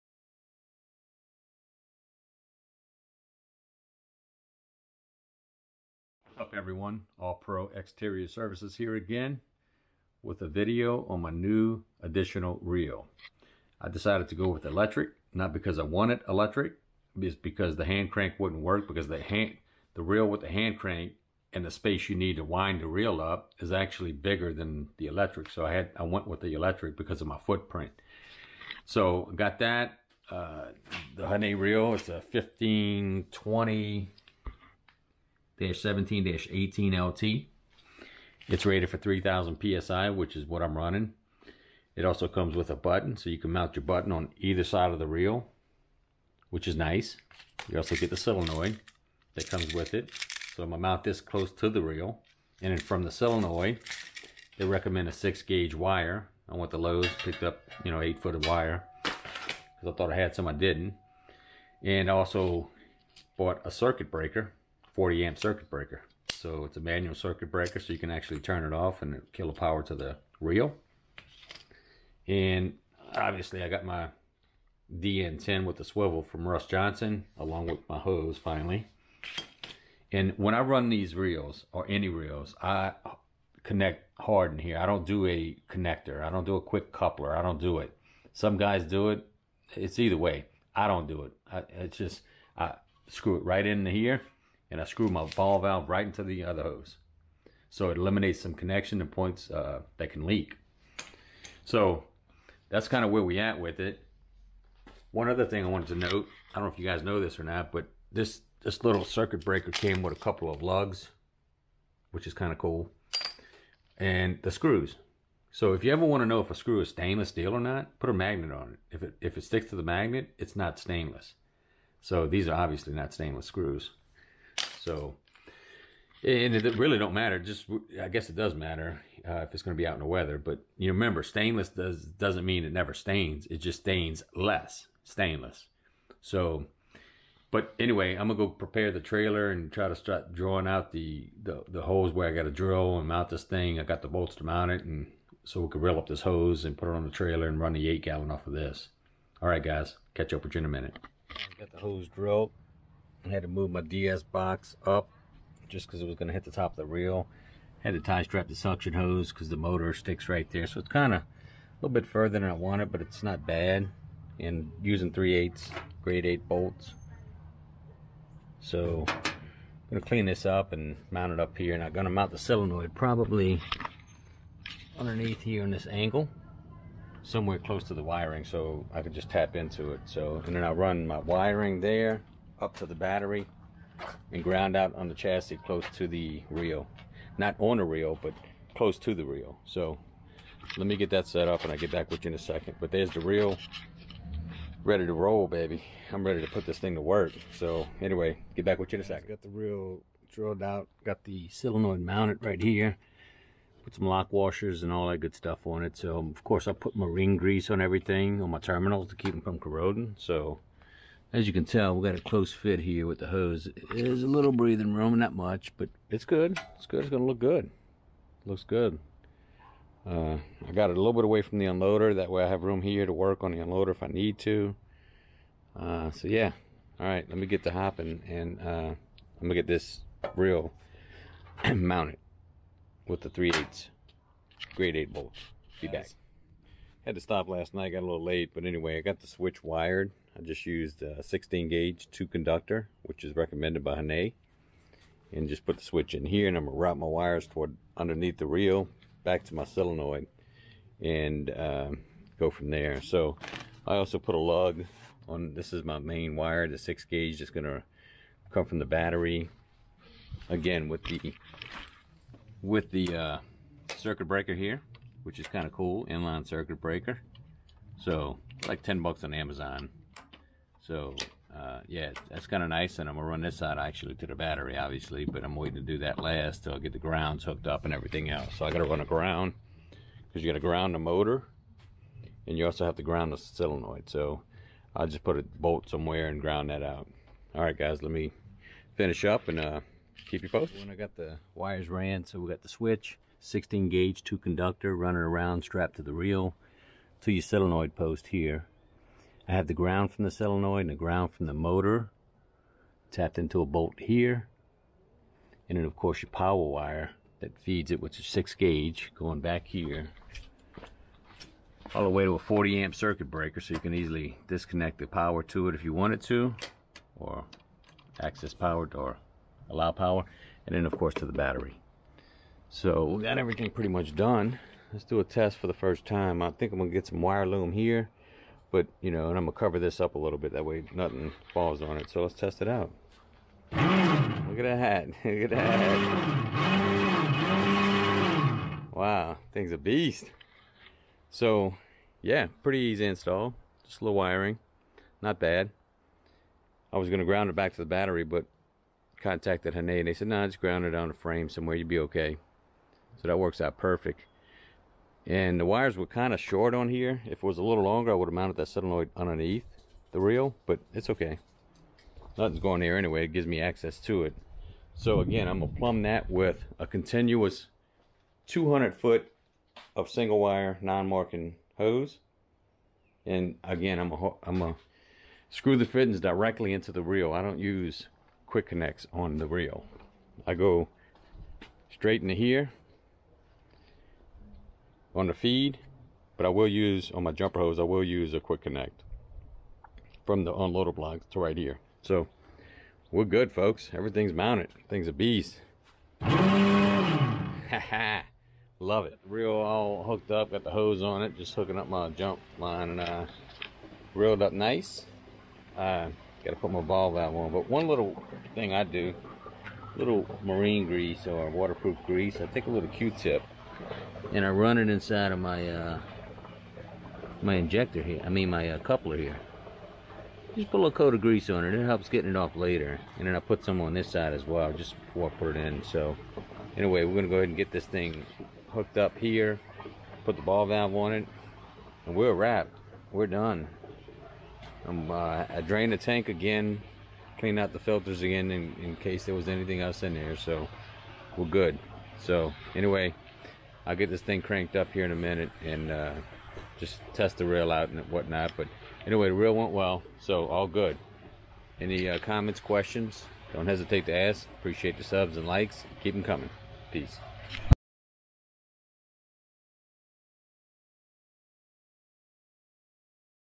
Hannay Electric Reel Addition
I installed my Hannay electric reel and tested it out.